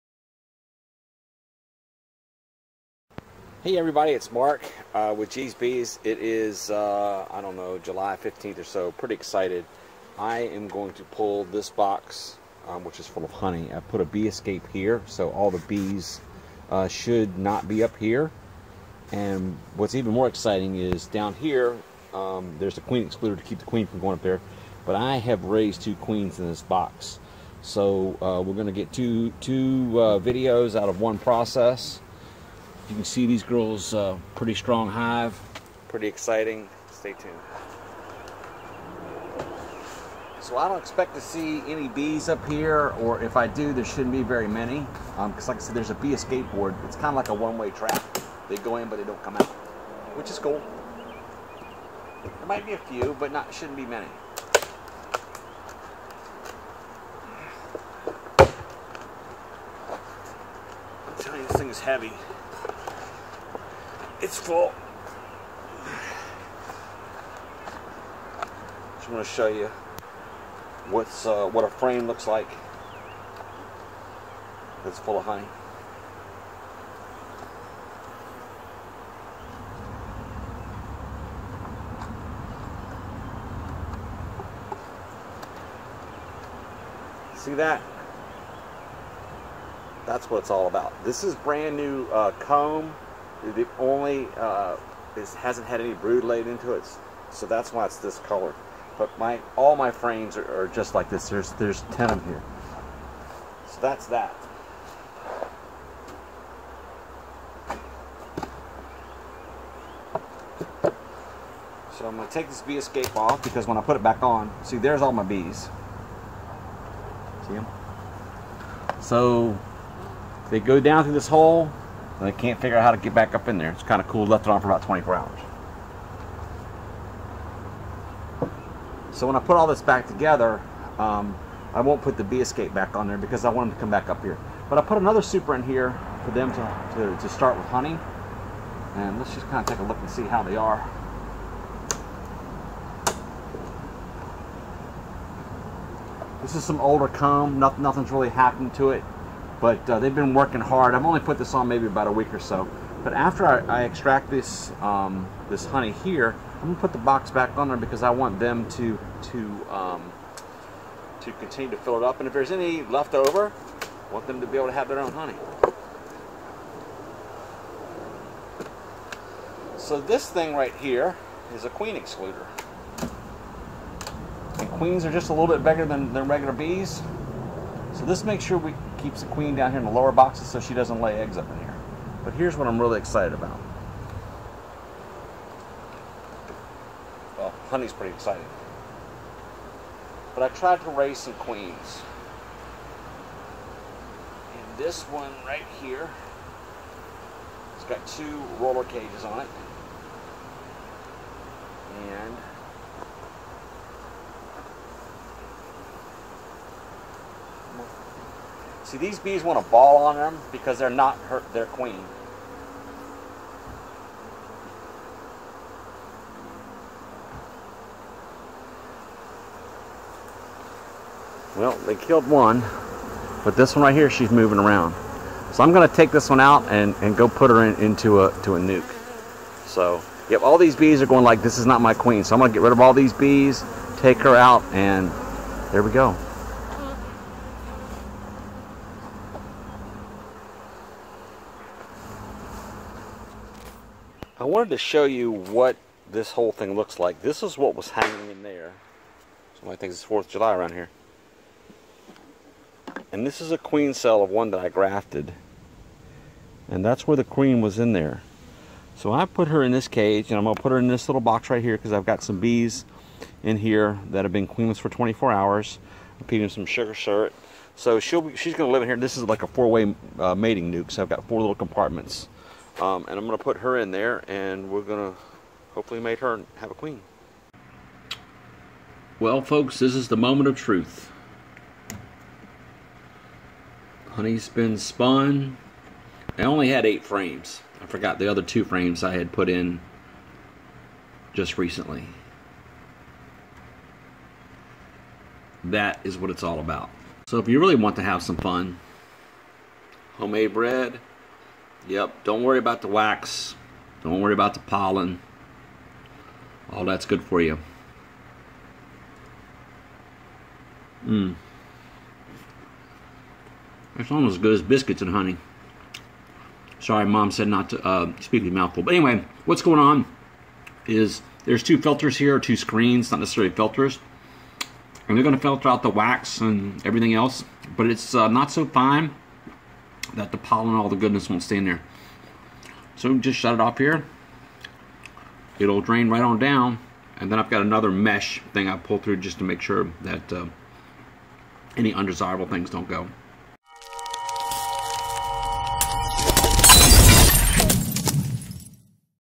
Honey Pull and a Grafted Queen
Pulling honey form a strong hive. Nothing better that honey that was just extracted.